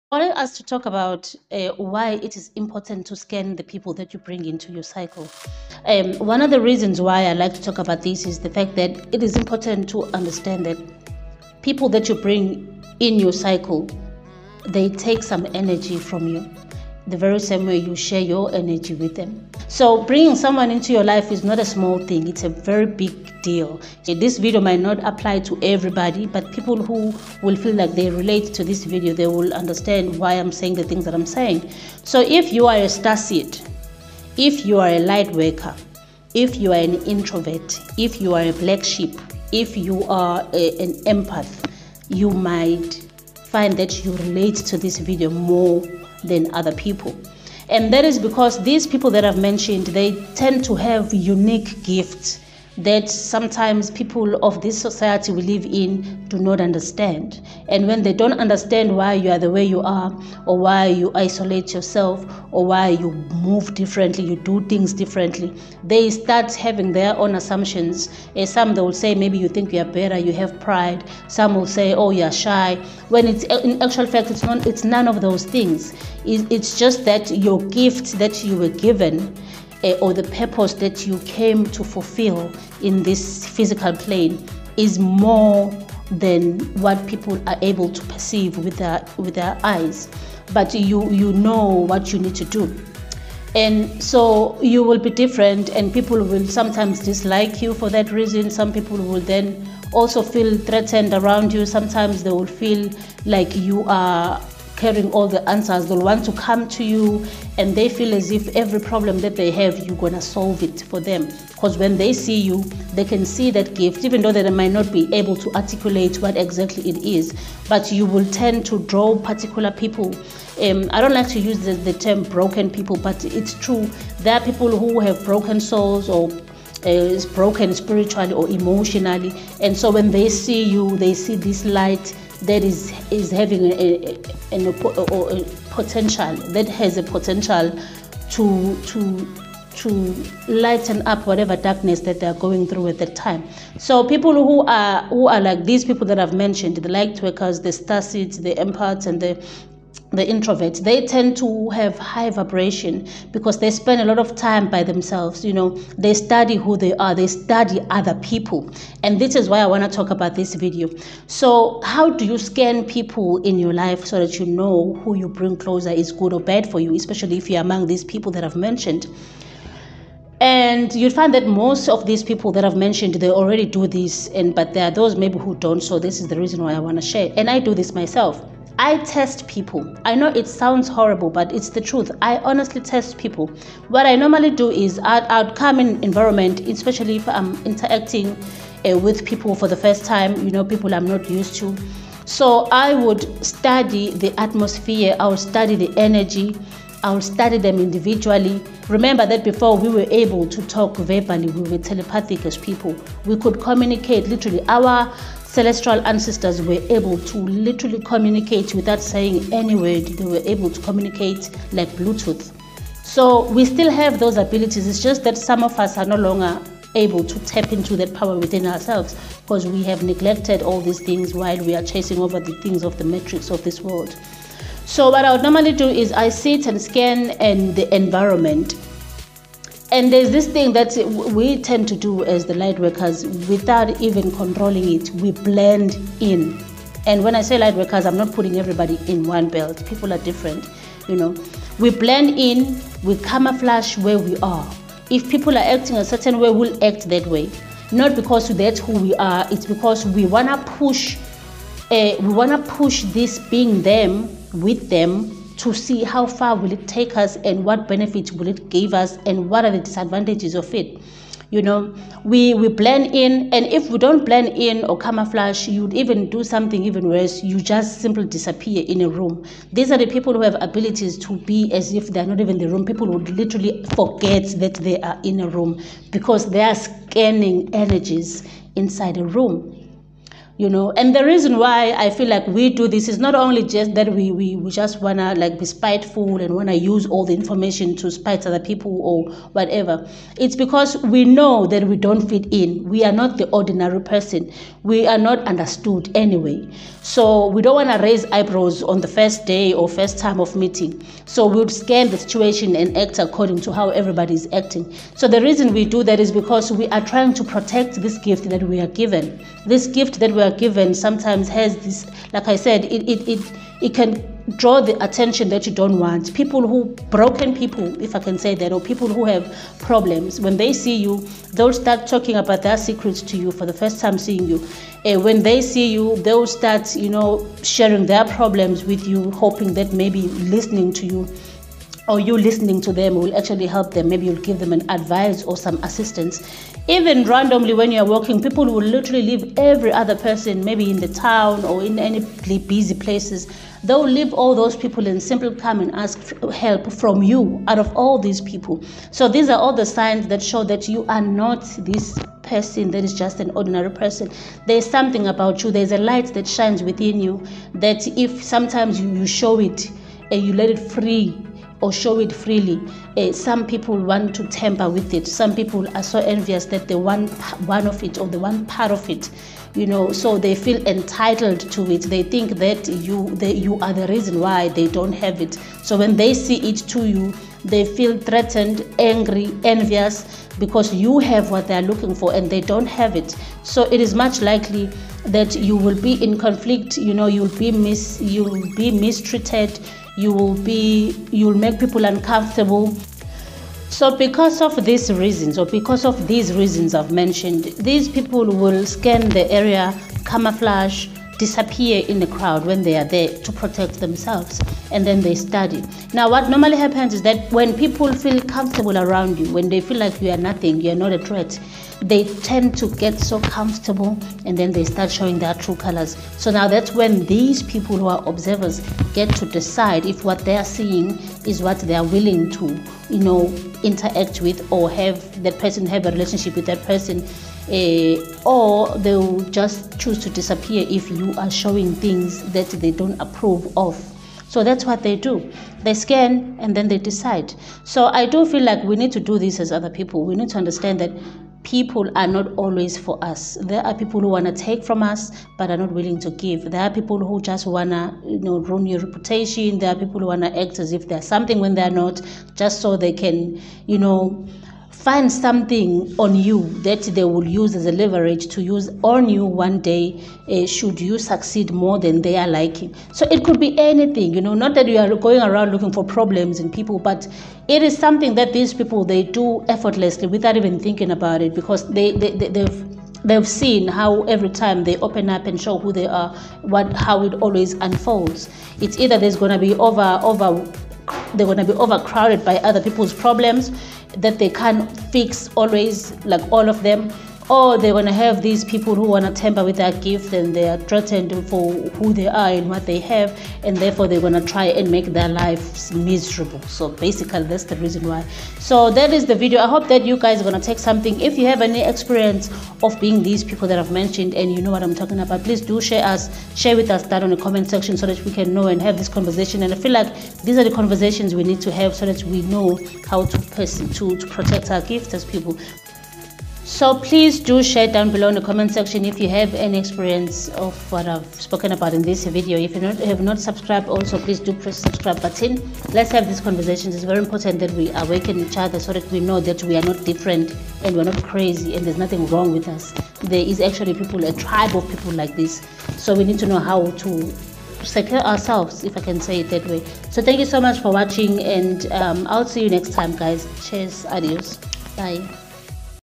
Reasons why you must test people
In this video I am going in depth about the importance of testing people who are in your inner cycle or those who wish to enter into it. It is important that we know the value people bring in our lives so we can be able to interact with them accordingly.